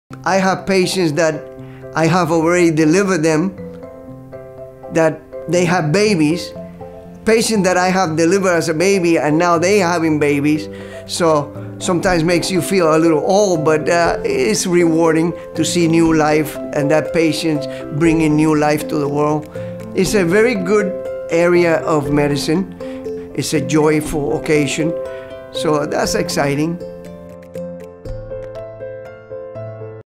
Years of Experience Matter When Looking For The Right OB
CarePoint Health OB-GYN Raul Aguilar has patients he delivered as babies who are now having babies. Find experienced, caring OBs to guide you